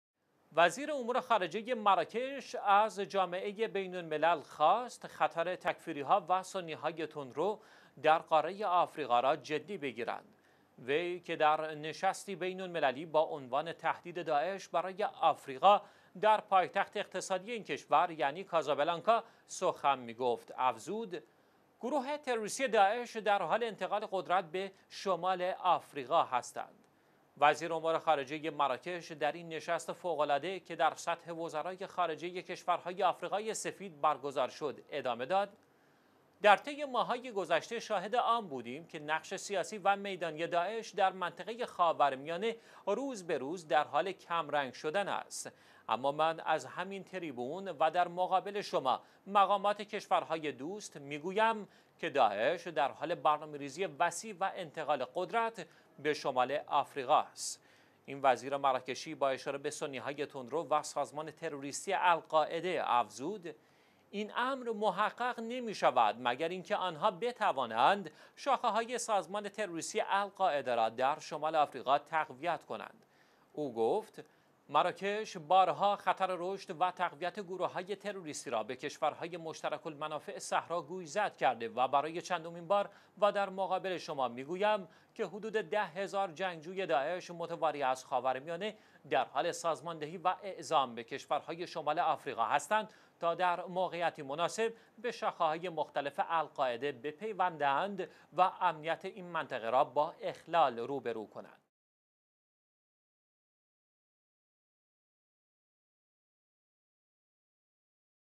وزیر امور خارجه مراکش: داعش در حال انتقال قدرت به شمال آفریقا است
وزیر امور_خارجه مراکش از جامعه بین الملل خواست تا خطر تکفیری ها و سنی های تندرو را در قاره آفریقا جدی بگیرند.

وی که در نشستی بین المللی تحت عنوان «تهدید داعش برای آفریقا» در پایتخت اقتصادی این کشور یعنی کازابلانکا سخن می گفت، افزود: گروه تروریستی داعش در حال انتقال قدرت خود به شمال آفریقا است.

وزیر امور خارجه مراکش، در این نشست فوق العاده که در سطح وزرای خارجه کشورهای آفریقای سفید برگزار شد، ادامه داد: در طی ماه های گذشته شاهد آن بودیم که نقش سیاسی و میدانی داعش در منطقه خاورمیانه روز به روز در حال کمرنگ شدن است، اما من از همین تریبون و در مقابل شما مقامات کشور های دوست می گویم که داعش در حال برنامه ریزی وسیع و انتقال قدرت به شمال آفریقا است.

این وزیر مراکشی با اشاره به سنی های تندرو و سازمان نروریستی القاعده، افزود: این امر محقق نمی شود مگر آنها بتوانند شاخه های سازمان تروریستی القاعده را در شما آفریقا تقویت نمایند.

او در ادامه بیان کرد: مراکش بارها خطر رشد و تقویت گروه های تروریستی را به کشور های مشترک المنافع صحرا گوشزد کرده و برای چندمین بار و در مقابل شما می گویم که حدود 10 هزار جنگجو متواری داعش از خاورمیانه در حال سازماندهی و اعزام به کشور های شمال آفریقا هستند تا در موقعیتی مناسب به شاخه های مختلف القاعده پیوسته و امنیت این منطقه را با اخلال رو به رو کنند.